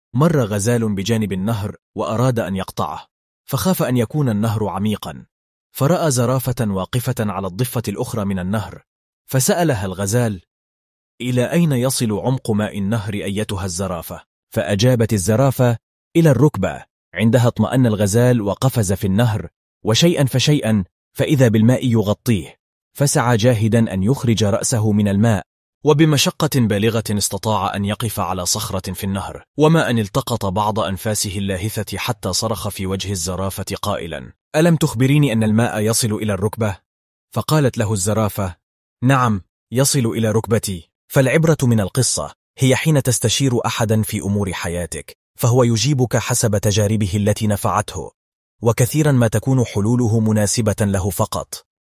قصة الغزال والزرافة، عبرة ونصيحة في دقيقة
مرّ غزال بجانب النهر وأراد أن يقطعه، فخاف أن يكونَ النهرُ عميقا ، فرأى زرافةً واقفةً على الضفة الأخرى من النهر، فسألَها الغزال:  إلى أين يصل عمقُ ماء النهر؟ فأجابت الزرافة: إلى الرُّكبة! عندها اطمأن الغزال وقفز  في النهر ، وشيئا فشيئا ،فإذا بالماء يُغطّيه!! فسعى جاهداً أن يُخرجَ رأسهُ من الماء ! وبمشقّةٍ بالغةٍ استطاعَ أن يقفَ على صخرةٍ في النهر، وما أن التقطَ بعضَ أنفاسهِ اللّاهثةِ حتى صرخَ في وجهِ الزّرافة قائلاً: ألم تُخبريني انّ الماءَ يصل إلى الركبة؟ فقالت له الزَّرافة: نعم يصلُ إلى رُكبتي!
فالعبرة من القصة هي حين تستشيرُ أحداً في أمور حياتك فهو يُجيبكَ حَسبَ تجاربهِ التي نفعتهُ! وكثيراً ما تكونُ حلولهُ مناسبةً لهُ فقط!